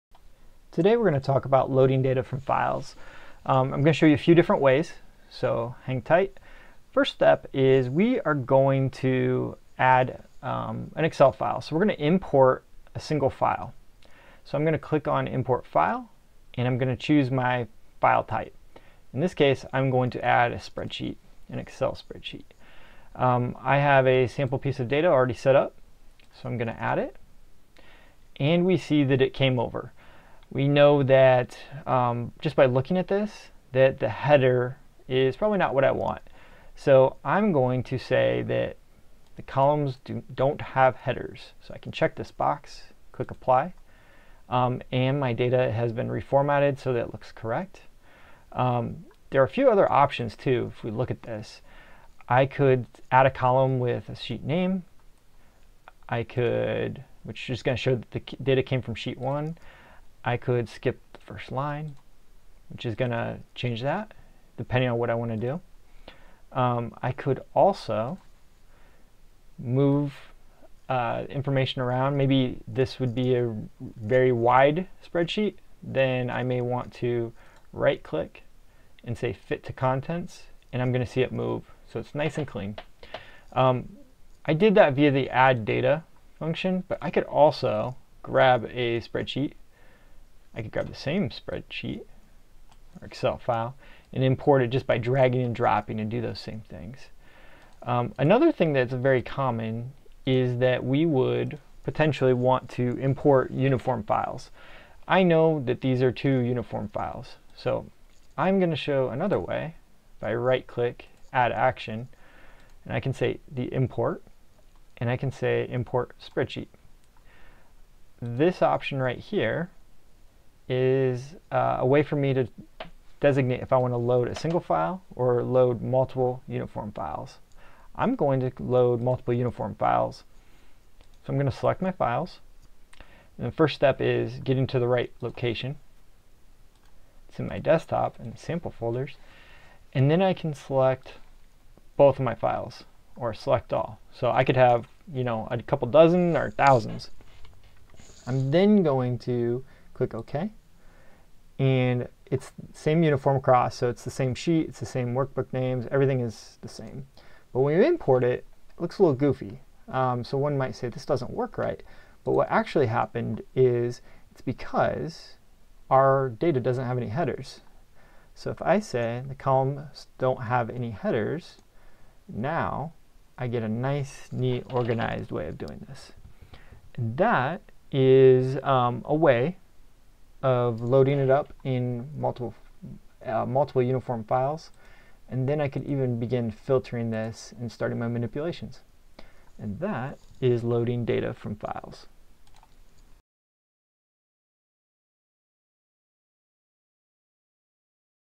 Loading Data From Files (Spreadsheets) in EasyMorph
Demonstration of how to load a single Excel spreadsheet or multiple uniform Excel spreadsheets and append them automatically in EasyMorph

The video is created by EasyMorph user Adam Beltz.